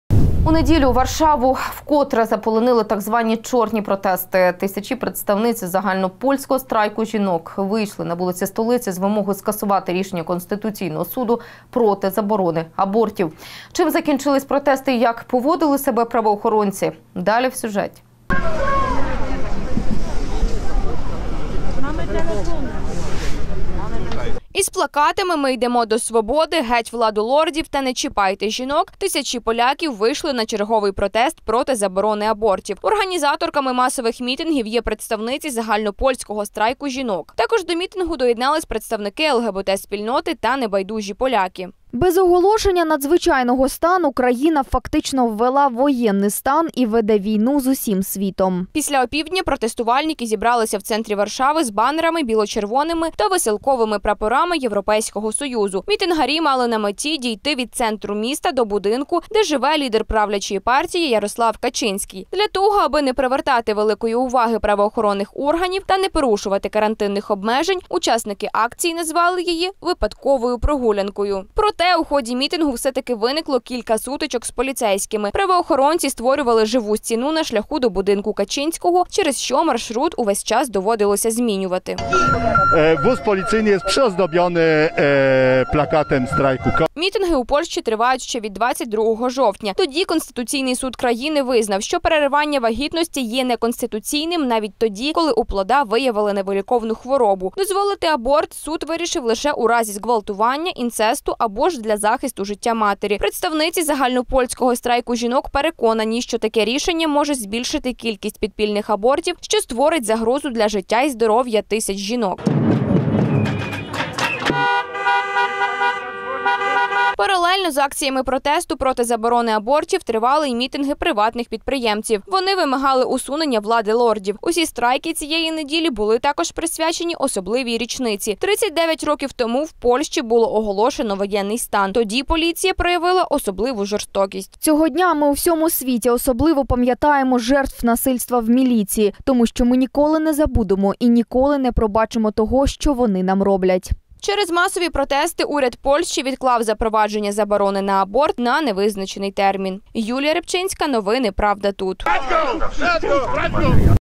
Не чіпайте жінок!
У Польщі тривають антиурядові протести. Мітингарі вимагають скасувати рішення Конституційного суду проти заборони абортів